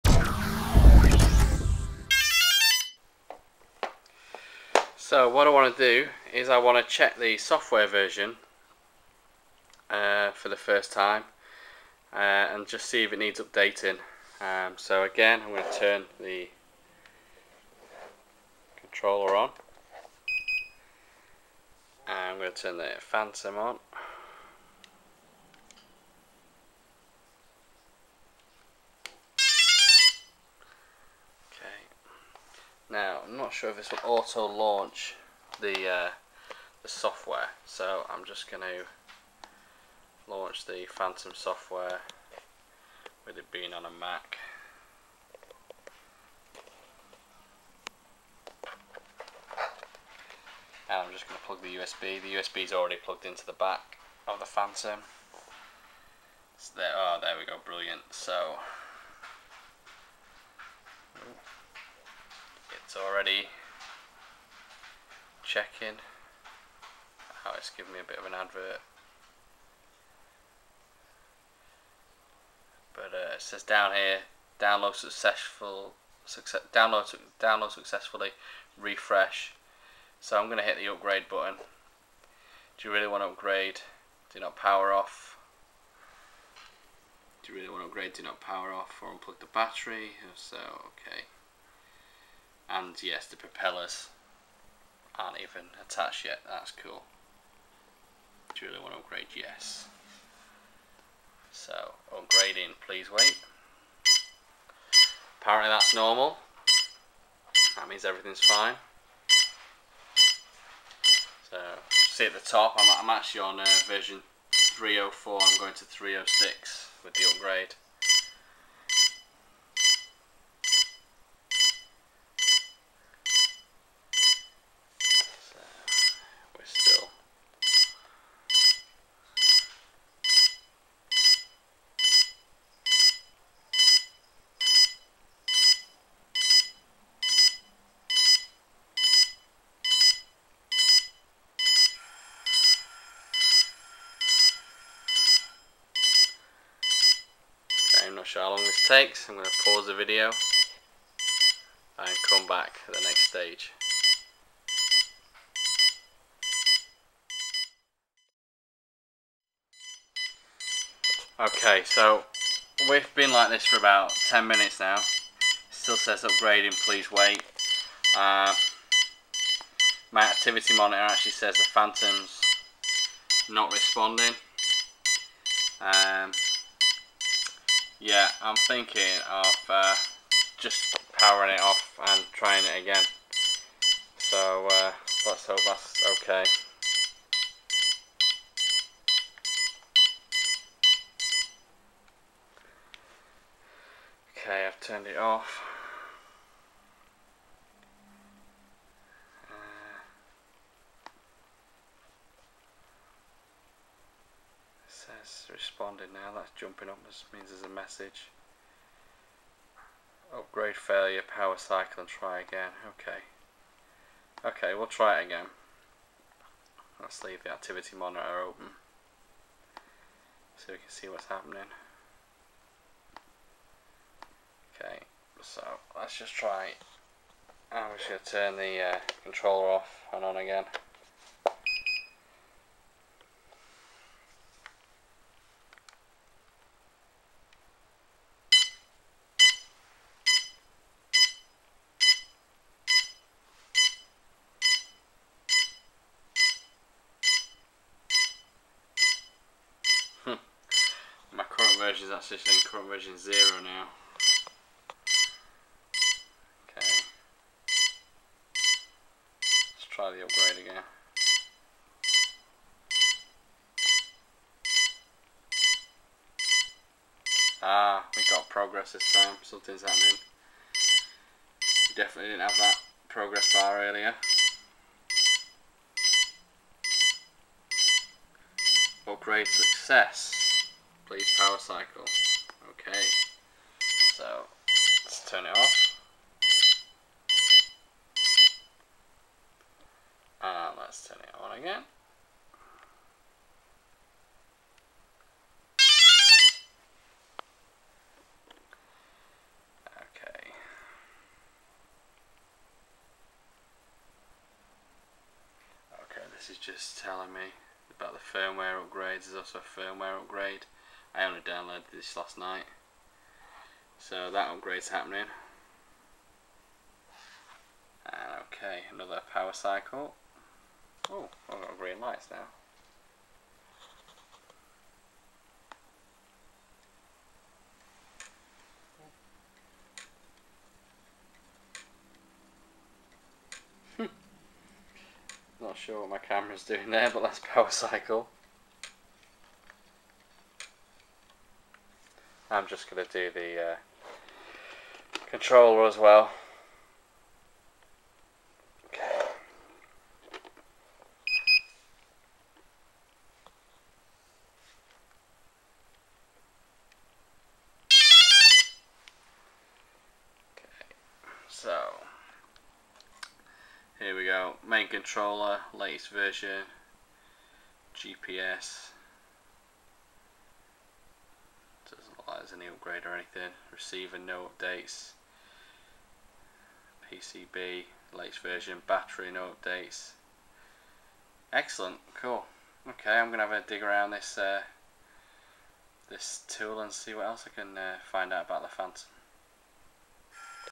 AirReal: software update error DJI Phantom 2 Vision + Update 3.06
AirReal: after receiving my 2nd hand DJI P2V+ I wanted to check and upgrade the software and also recalibrate the controller, the software was on 3.04 and there was an update for 3.06, but the update seemed to be stuck, even though the warnings say don't power cycle I took the risk and it seemed to pay off.